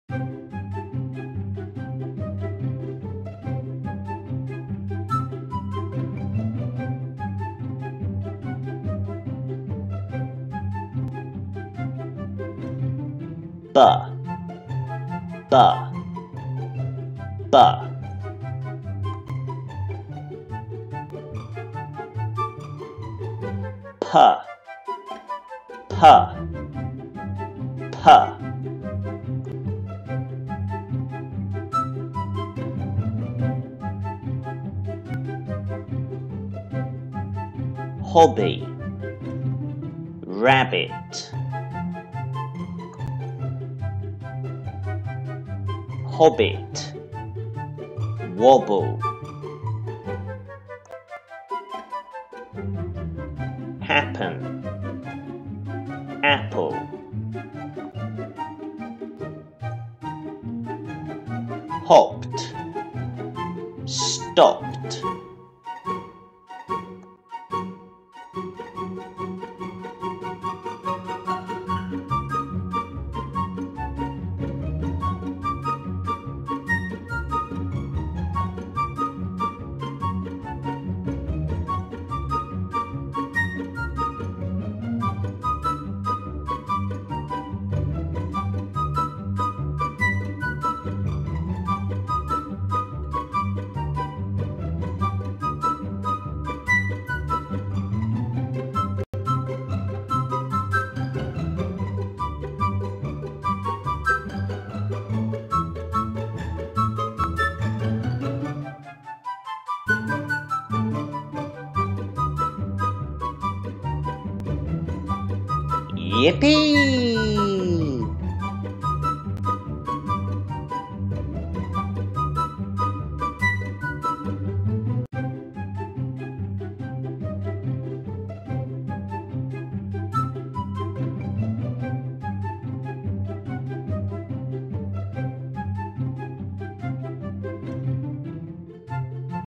EASY bb pp
This is the shorter, easier version of the bb and pp phonics video, including jaunty music. Good for foot-tapping, competitive classroom fun. Download the classroom word-list worksheets which accompany all the EASY version phonics videos